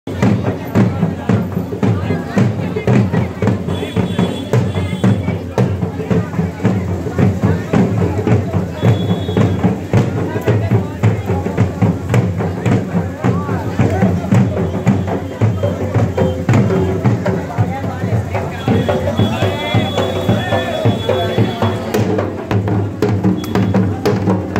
Live Munger Durga Visarjan || Jamalpur Durga Visarjan || Famous Visarjan Munger
Munger Durga Visarjan.
26th Oct 2020
Before Munger kaand.